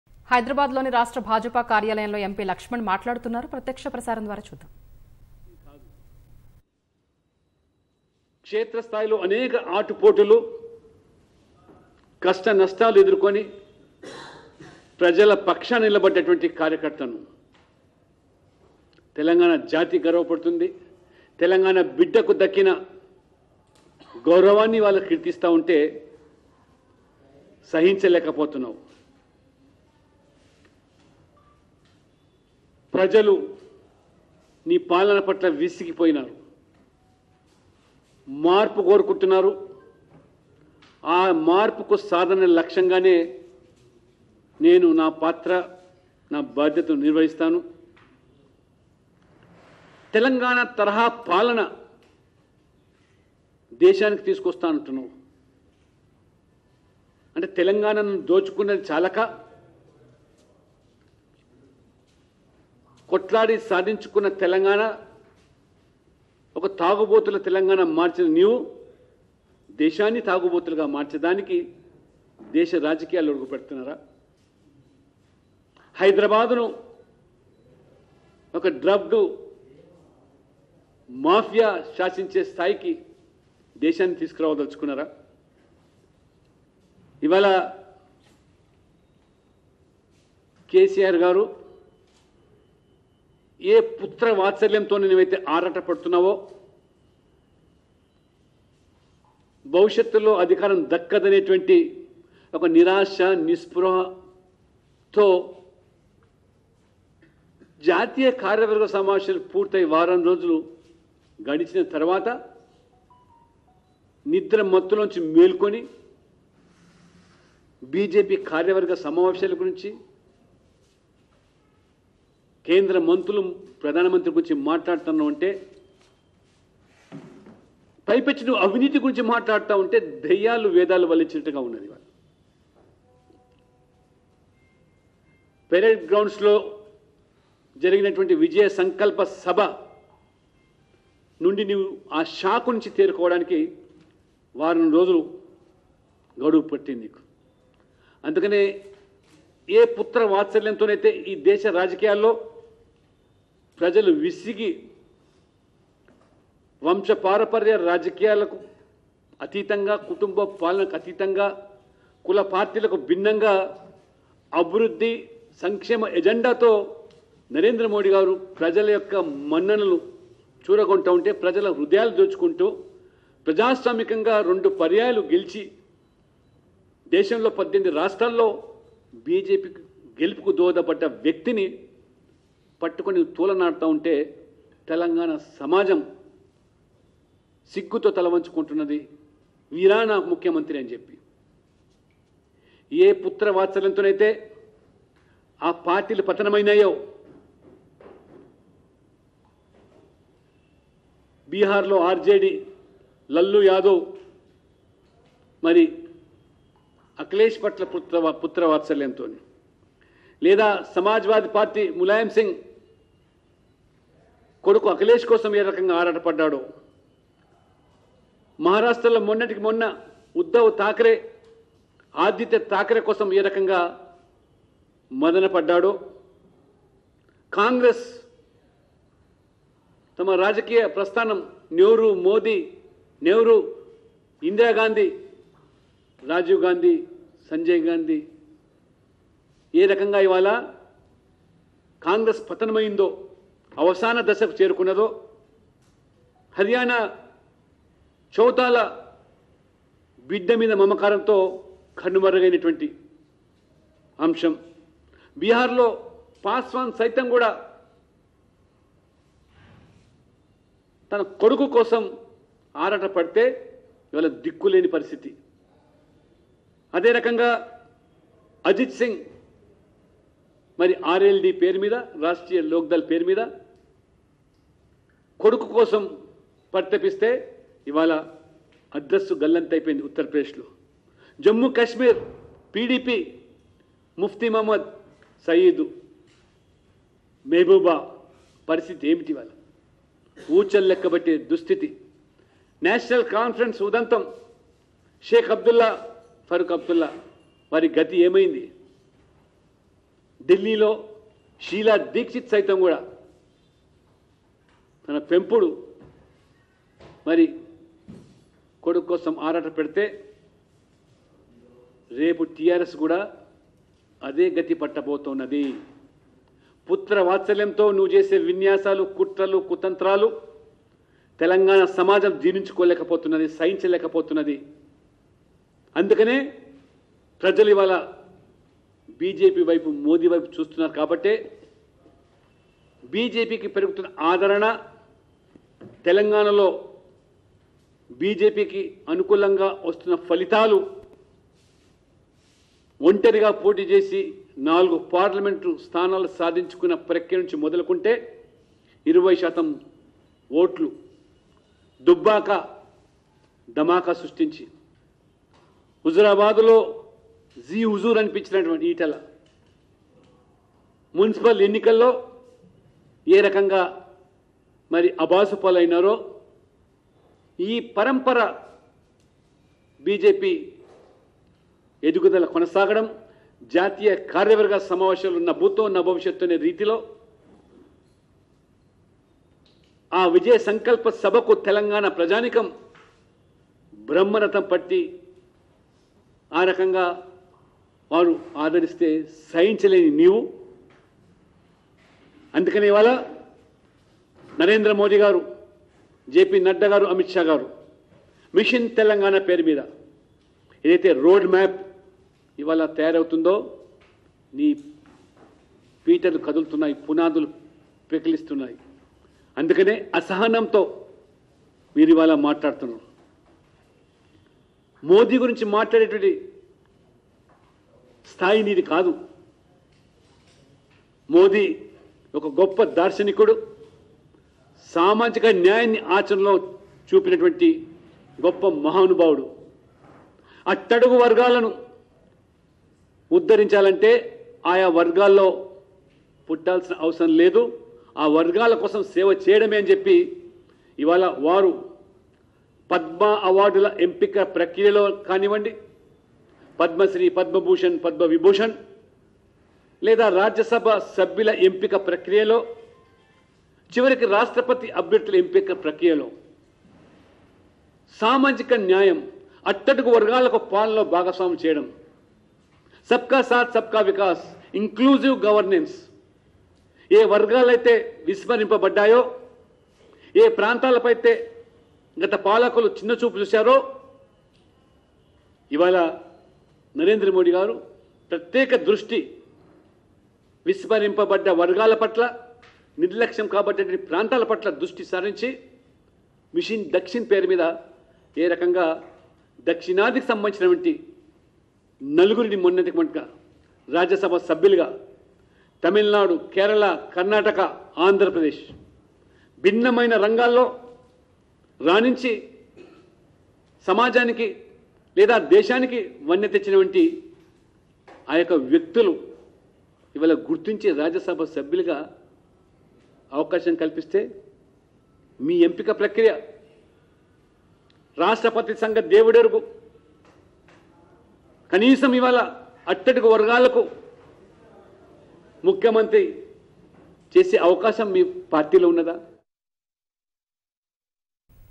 KCR's Words About Corruption Seem To Be Inspired By The Vedas | MP Laxman Press Meet
హైదరాబాద్ లోని రాష్ట్ర భాజపా కార్యాలయంలో ఎంపీ లక్ష్మణ్ మాట్లాడుతున్నారు.